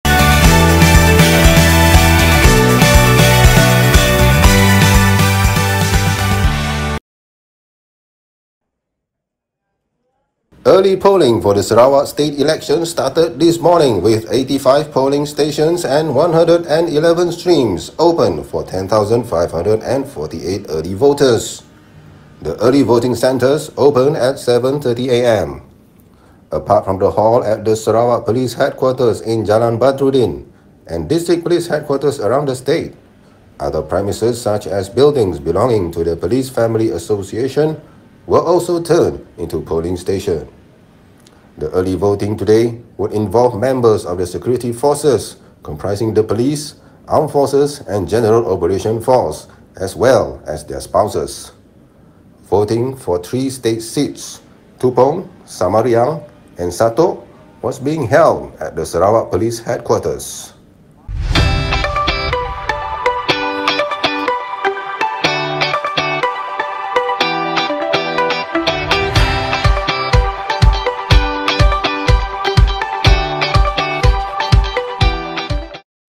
Early voting begins in Sarawak
Early polling for the Sarawak state election started this morning, with 85 polling stations and 111 streams opened for 10,548 early voters.
The early voting centres opened at 7.30am.

Apart from the hall at the Sarawak police headquarters in Jalan Badruddin here and district police headquarters around the state, other premises such as buildings belonging to the Police Family Association (Perkep) were turned into polling centres.

The early voting today would involve members of the security forces, comprising the Royal Malaysia Police (PDRM), Malaysian Armed Forces (ATM) and General Operations Force (PGA) as well as their spouses.

Voting for three state seats - Tupong, Samariang, and Satok - was being held at the Sarawak police headquarters here.